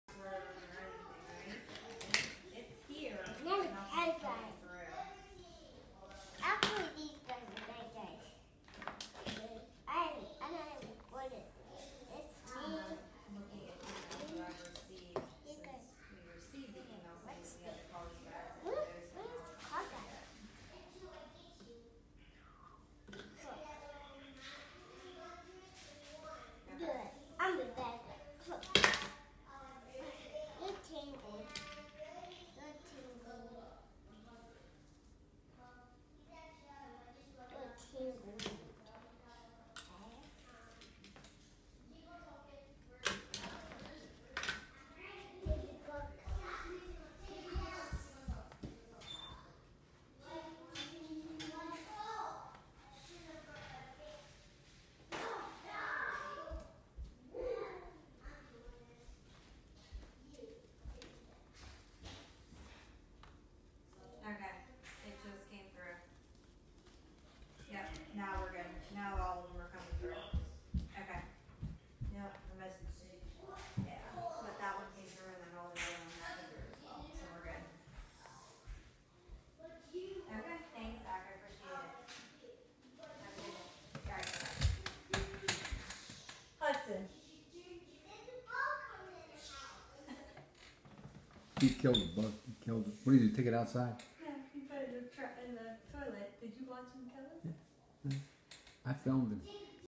7.16.2019 Hudson vs Fly
Hudson kills a fly